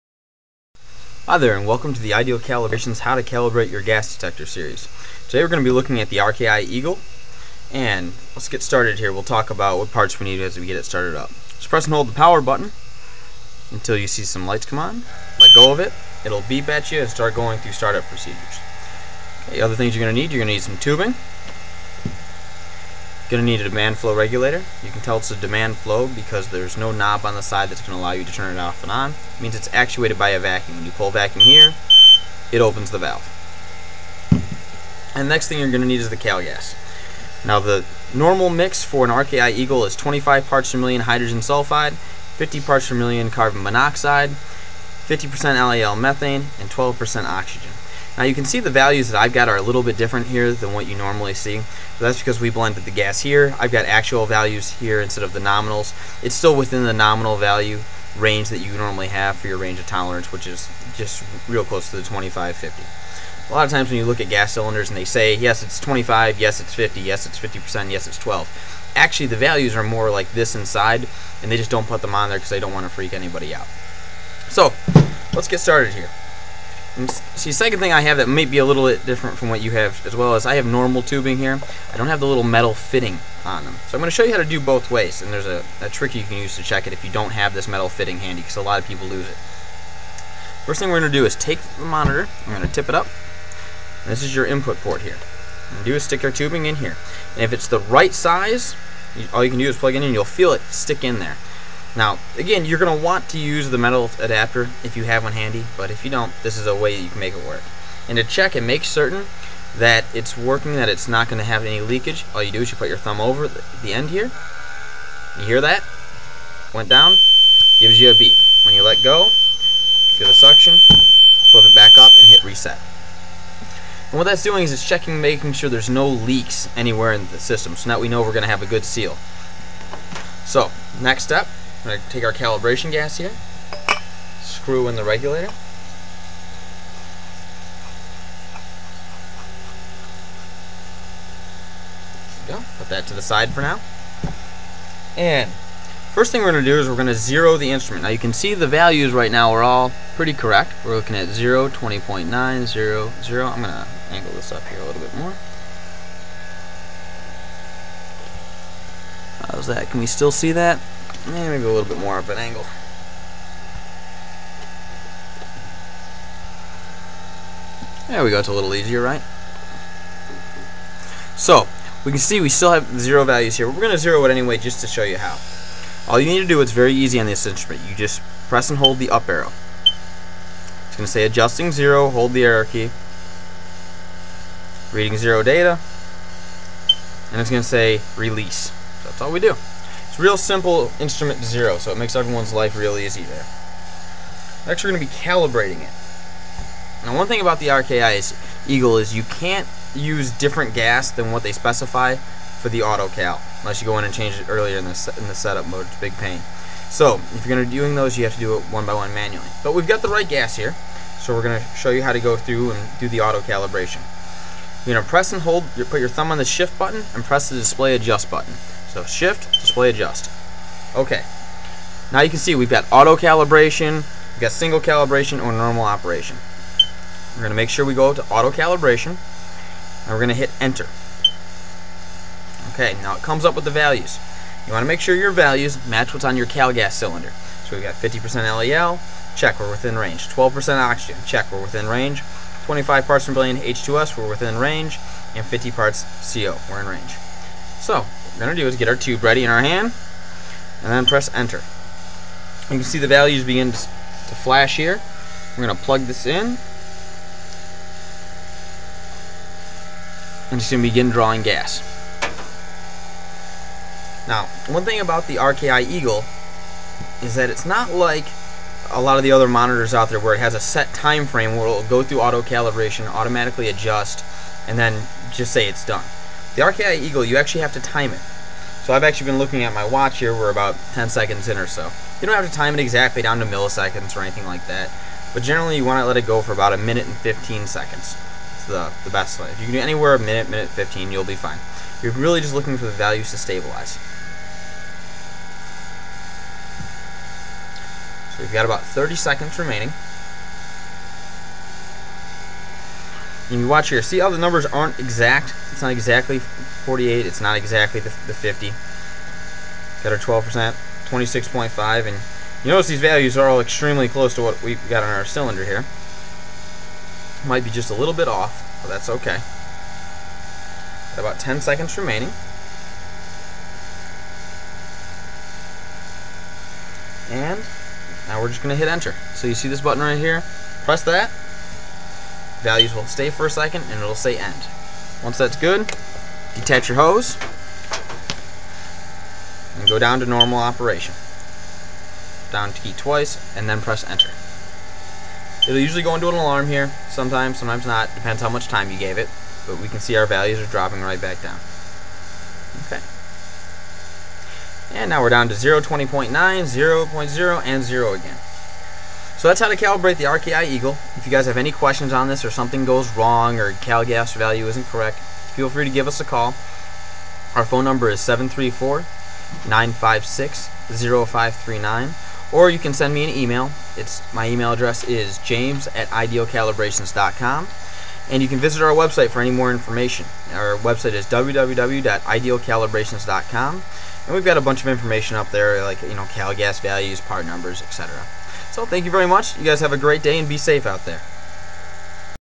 RKI Eagle Gas Calibration Guide by Ideal Calibrations
For more information regarding the RKI Eagle Gas Detector (including helpful part numbers) please

This is a video in the How To Calibrate Your Gas Monitor series by Ideal Calibrations.  All you need is trusted calibration gas (of course we recommend Ideal Gases brand), a cal cup, a regulator, and tubing.

If you have a problem during your calibration, give us a call or send it in for repair here:

RKI Eagle Calibration Guide:
Want to calibrate the RKI Eagle? Here's how:
1. Turn the RKI Eagle on in normal reading mode. Do this by pressing and Holding the power button wait for light to come on then release.
2. Once the RKI Eagle Gas is in normal reading mode, you need to send it into zero mode. Do this by pressing and holding the air/up button.
3. This will send the monitor into zeroing mode. Now is a good time to set the monitor down, and grab your calibration gas, regulator, and calibration adapter.
4. Connect your demand flow regulator.
5. After the RKI Eagle is zeroed, press the shift key and then press the display adjust key at the same time. This will bring up a menu, select auto calibration. This will send your monitor into calibration mode, attach the tubing to the hose barb on your regulator. Make sure the tubing is also connected to your monitor.
6. Make sure your calibration gas values on the screen match what is on your calibration gas cylinder!
7. Once the RKI Eagle has reached it's calibration values, press enter, disconnect your tubing and select normal operation.
8. Congratulations! You have officially calibrated your RKI Eagle gas monitor. Make sure to record these readings in your calibration log!

Calibration Gas for the RKI Eagle is:
25ppm Hydrogen Sulfide
50ppm Carbon Monoxide
50% LEL Methane
12.0% Oxygen
Nitrogen Balance

We'd be happy to answer any questions you have.